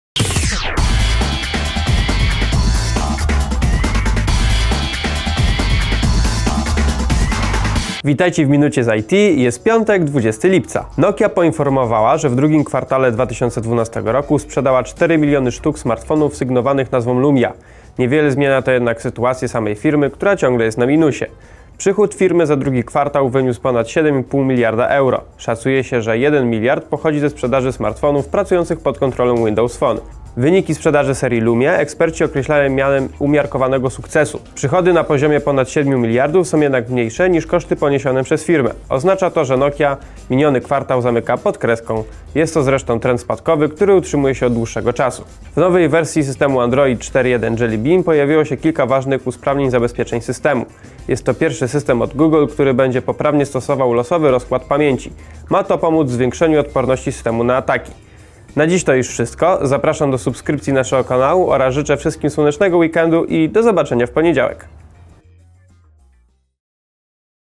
Minuta z IT - 20.07.2012
W dzisiejszej Minucie z IT: Nokia sprzedaje ale niewystarczająco oraz lepsze zabezpieczenia przed atakami w nowym Androidzie.